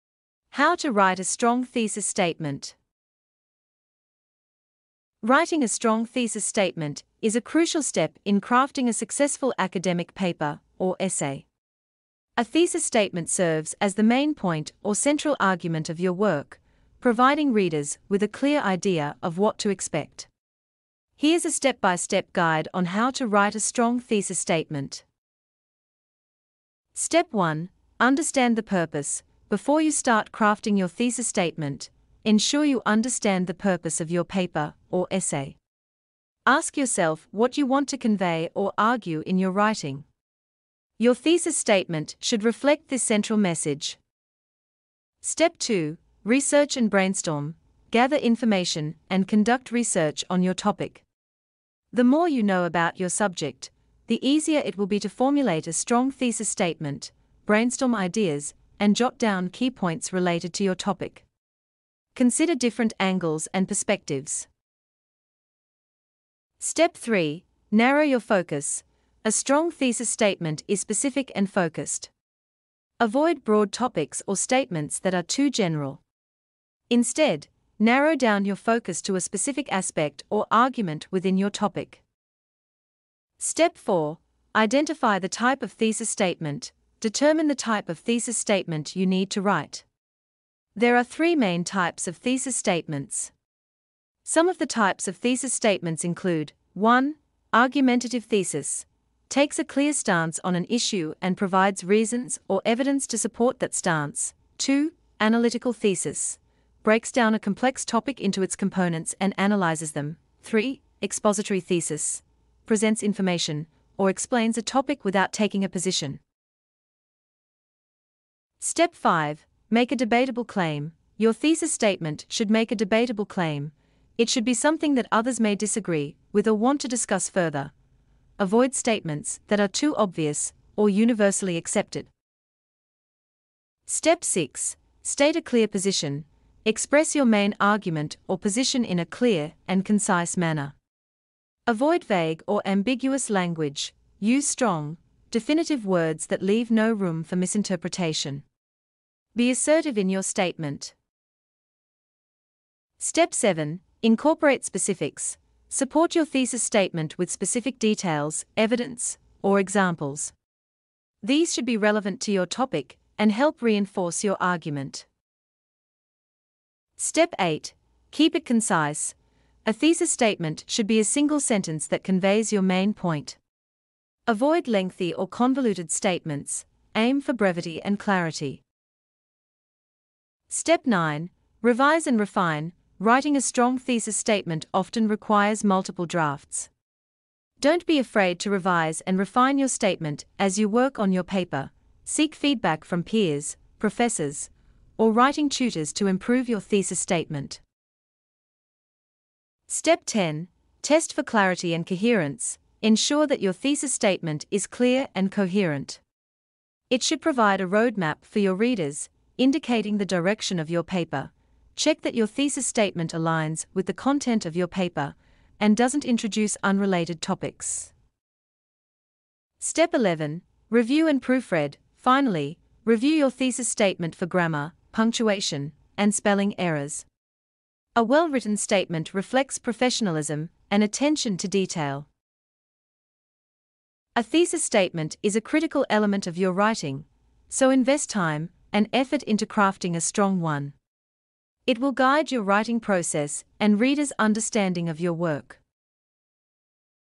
How to Write a Strong Thesis Statement
🔥🎇Check out our video on How to Write a Strong Thesis Statement 💪📝

Are you struggling to craft a thesis statement that truly stands out in your essays and research papers? Look no further! In this comprehensive video tutorial, we'll guide you through the step-by-step process of writing a strong thesis statement that will impress your instructors and elevate the quality of your academic work.

To get direct to the point you are more interested in use the time code below➡⏲:

00:00:00 Intro
Introduction on what is a strong thesis statement and how to write a strong thesis statement.✍🏻💪

00:00:25 Step 1
Before you start writing your thesis statement, ensure you understand the purpose of your paper or essay.

00:00:44 step 2
Gather information and conduct research on your topic

00:01:06 step 3
Narrow your focus and avoid broad topics or statements that are too general, focus to a specific aspect

00:01:23 step 4
Determine the type of thesis statement you need to write

00:01:57 step 5
Make you thesis statement a debatable claim

00:02:13 step 6
State a clear position and avoid a vague or ambiguous language.

00:02:32 step 7
Support your thesis statement with specific details evidence, or examples.

00:02:46 step 8
Your thesis statement should be concise, it should also convey your main point.

00:03:00 step 9
To come up with a strong thesis statement revise and refine your working thesis statement

00:03:19 step 10
Make sure you thesis statement is clear and coherent

00:03:39 step 11
Review your thesis statement to make sure it is free of errors.

00:03:53 Conclusion

00:04:08 The End 🔚